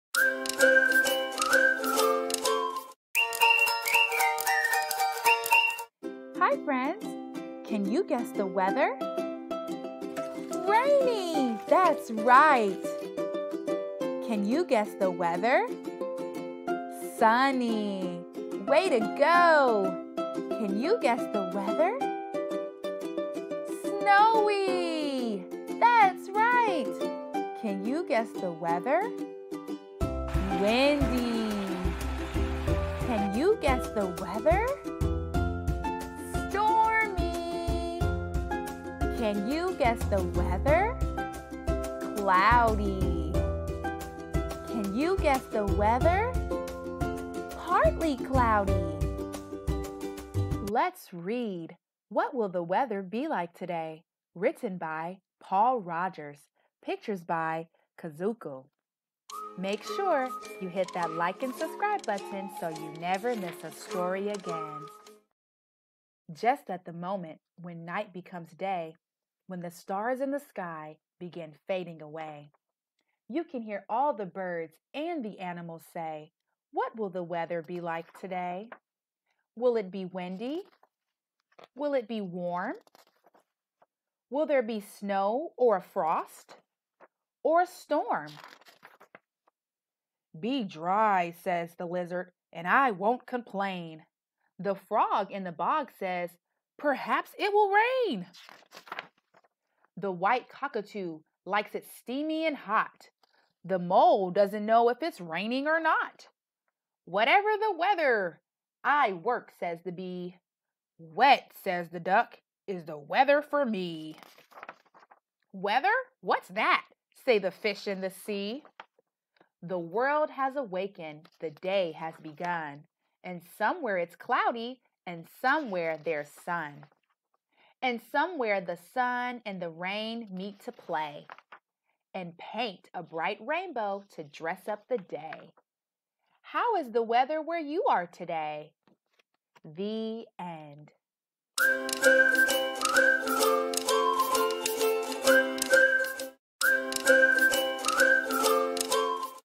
Weather for Kids | What Will the Weather Be Like Today?
"What Will the Weather be Like Today?"
"Whoosh! It's the Weather Book"
In this video, young children will have the opportunity to identify the different types of weather conditions. After the weather guessing game, children can engage in the reading of "What Will the Weather Be Like Today?" by Paul Rodgers and pictures by Kazuko. This book introduces early earth science concepts. Add this book to your library by clicking the link above.